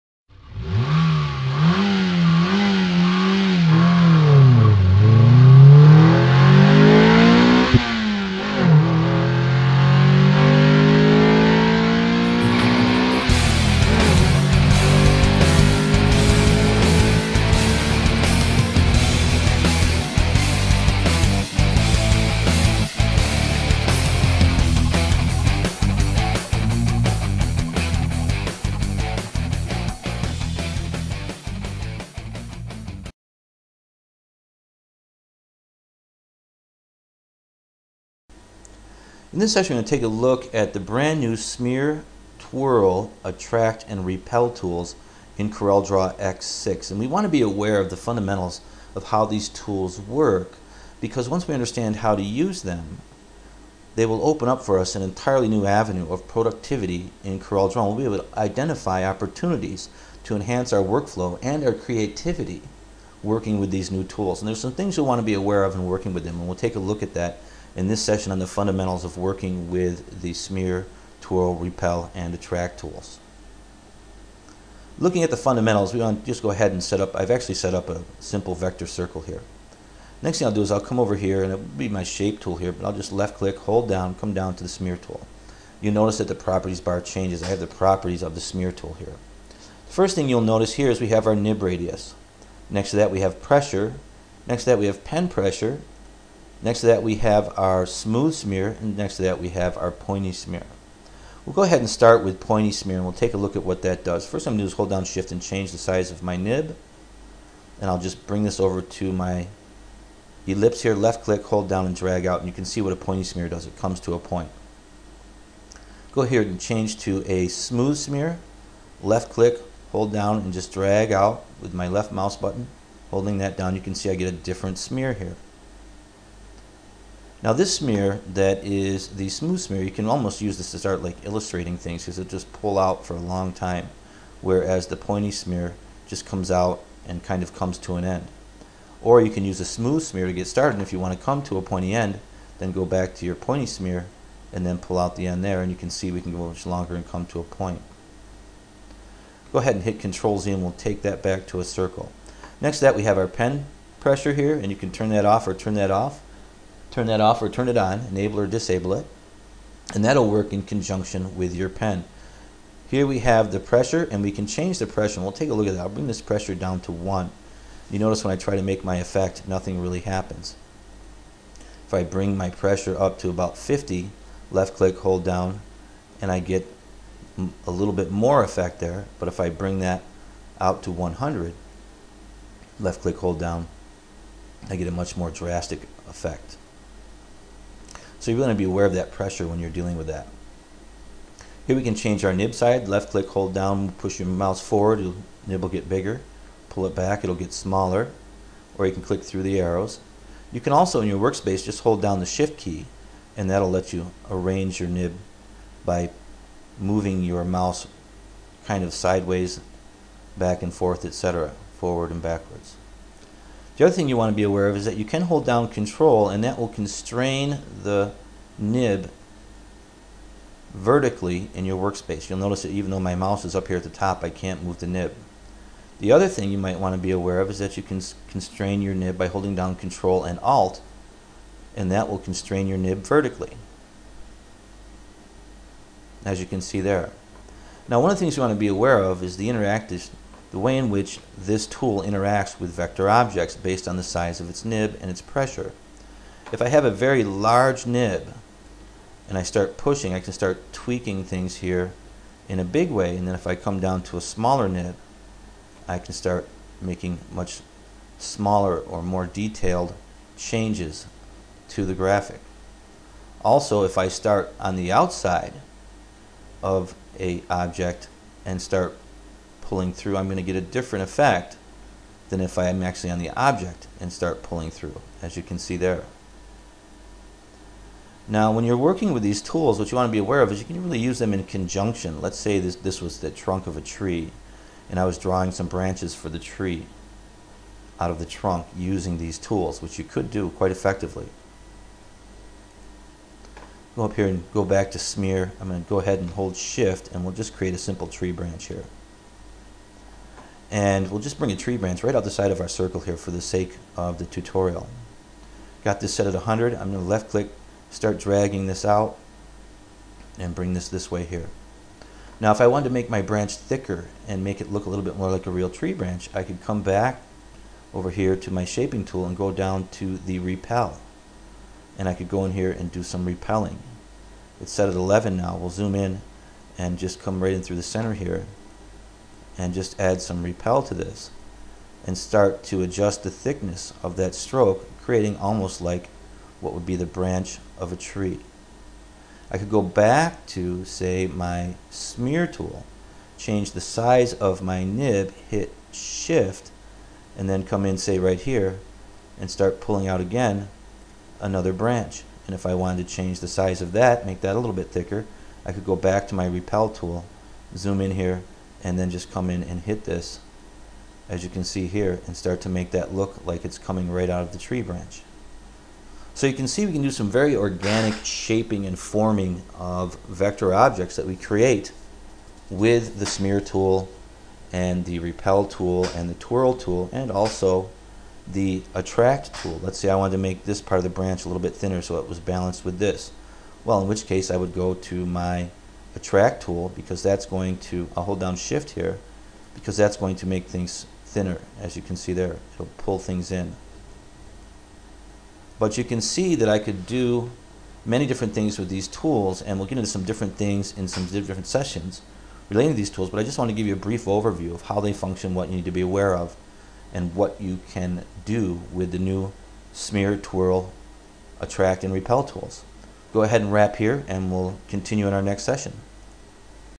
CorelDRAW X6 introduction to new vector tools.
This video tutorial will get you started with CorelDRAW X6 and the new vector smear, repel, attract and twirl tools.